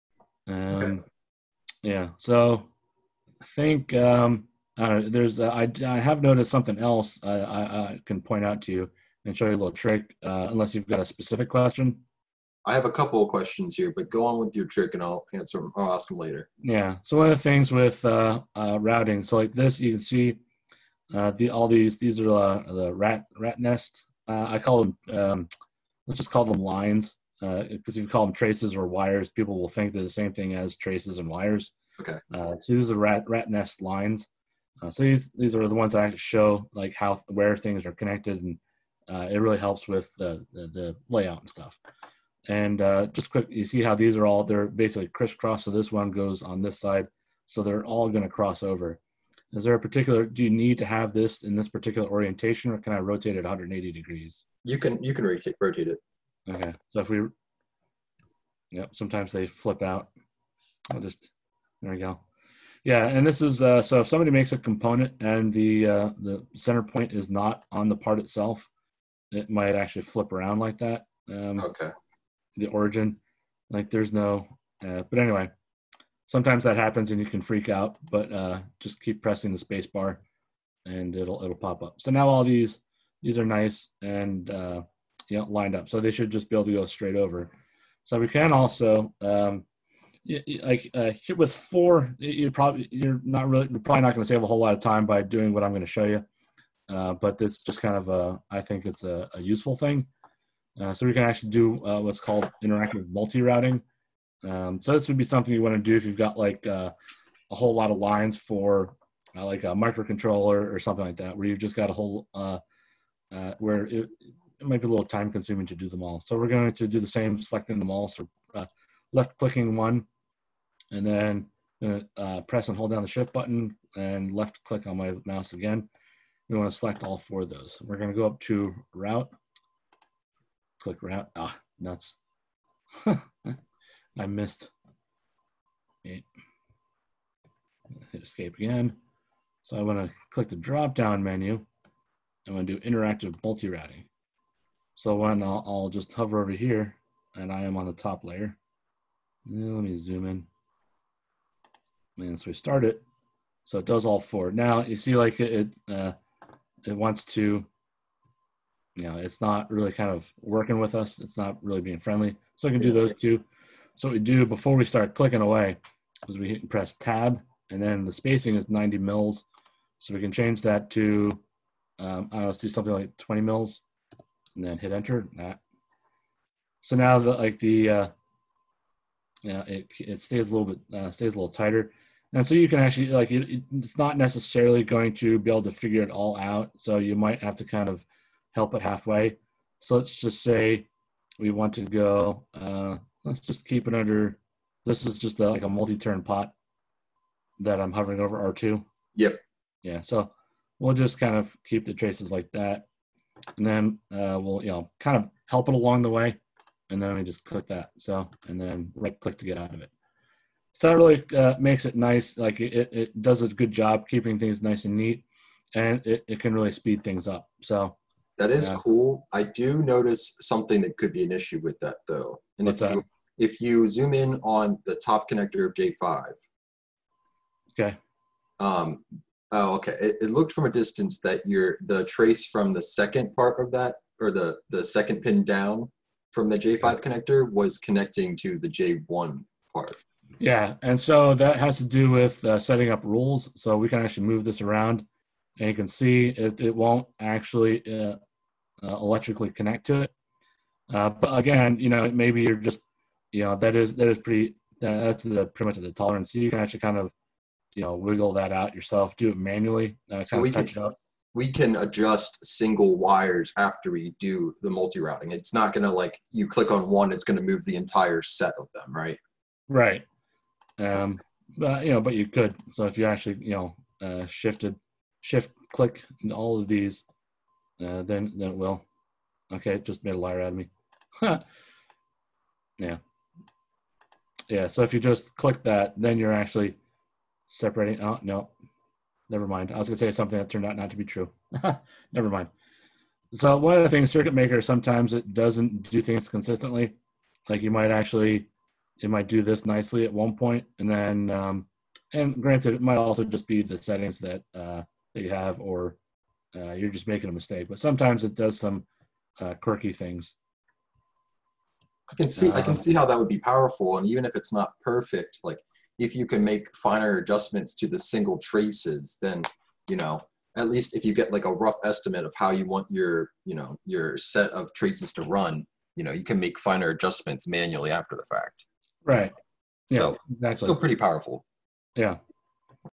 Circuitmaker: How To Use Interactive Multi-Routing Example
In this video we show how to route multiple traces simultaneously using the interactive multi-routing tool.